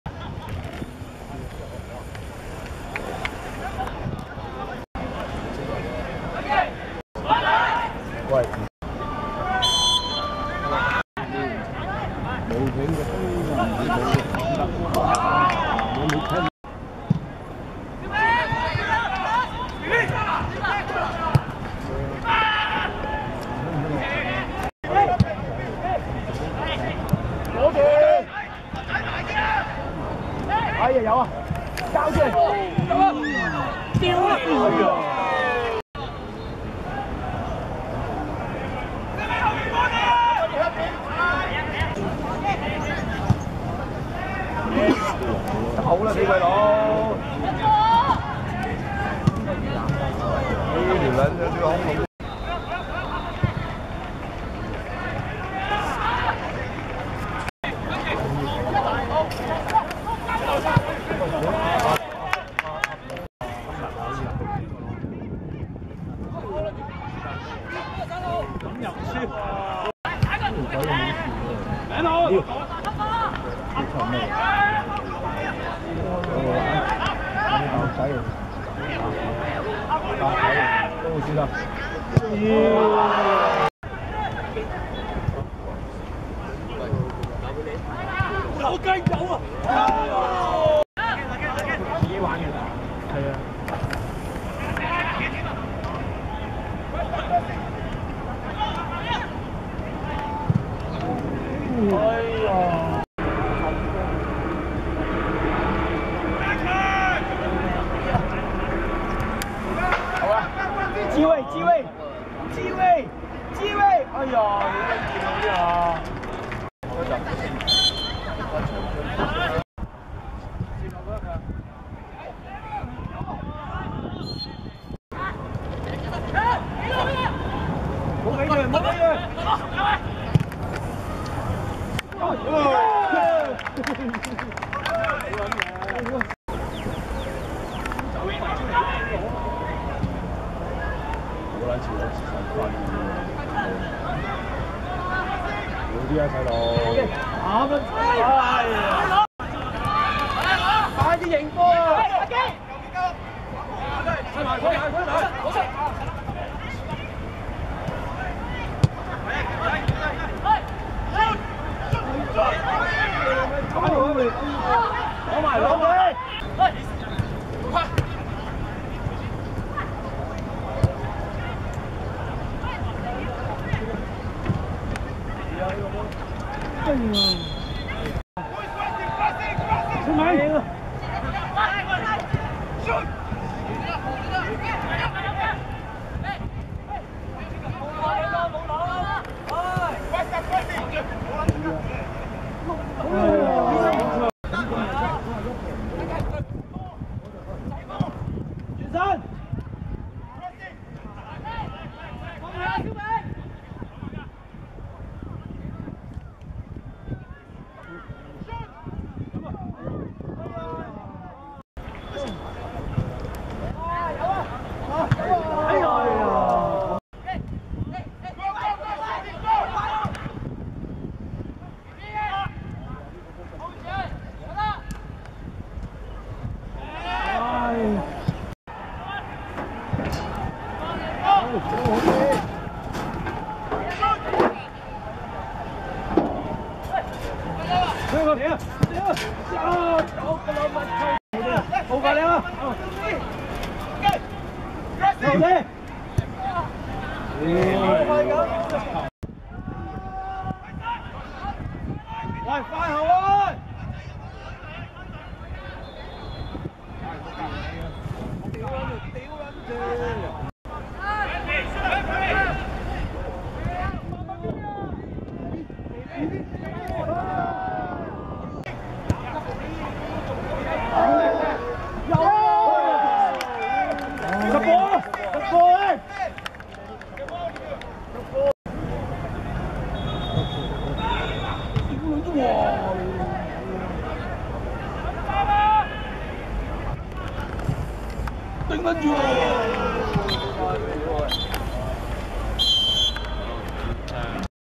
駿其天旭vs灣仔(2015.2.1.香港甲組足球聯賽)精華
2015.2.1.跑馬地6號塲 15:30 駿其天旭(藍) 2:0 灣仔(紅)